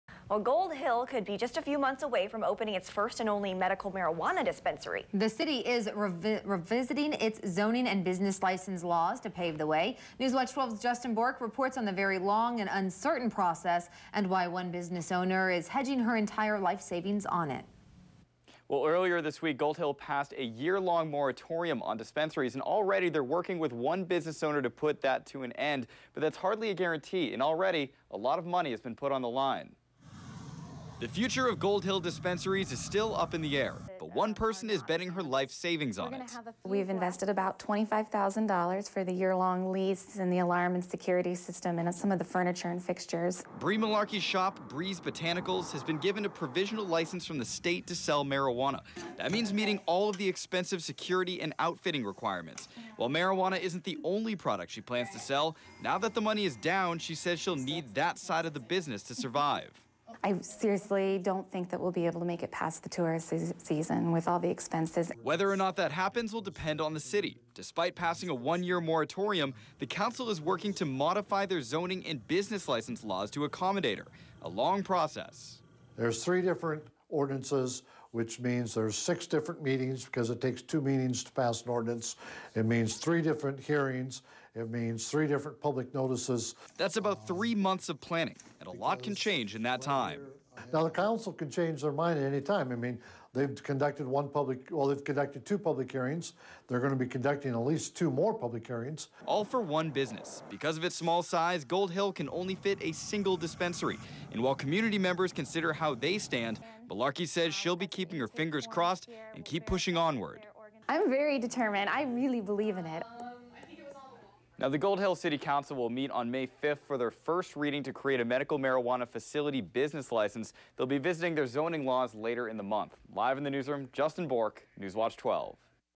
Dispensaries Still a Maybe in Gold Hill
Gold Hill could be just a few months away from opening its first and only medical marijuana dispensary. Despite passing a one-year moratorium this week, the city is revising its zoning and business license laws to pave the way.